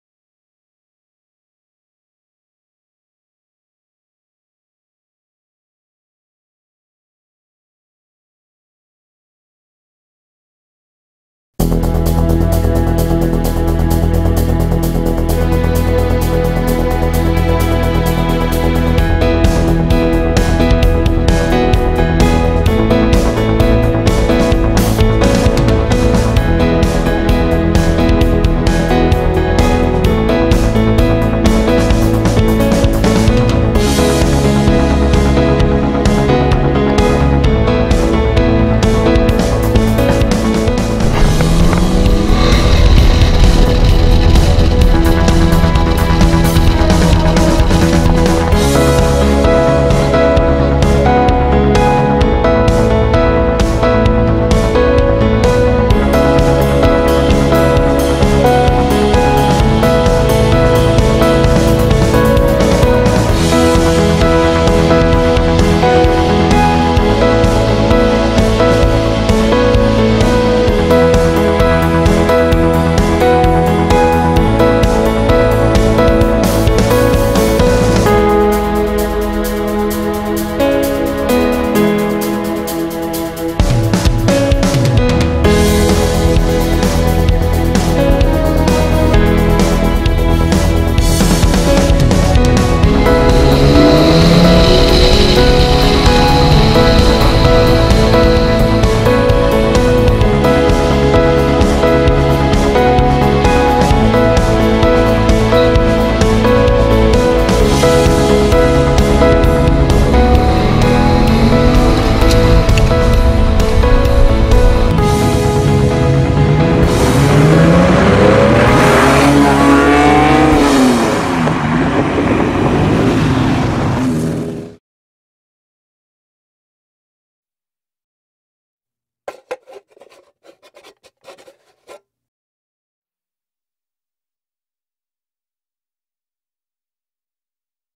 Universal Classic Cars at Goodwood's Italian Breakfast Club 2016
We take our Lamborghini Diablo VT to Goodwood's Italian Breakfast Club, October 2016.

Winner of the Diablo Tribute trophy at Salon Privé this year, this Diablo is one of only 400 produced and is presented in stunning dark metallic green, making it really stand out.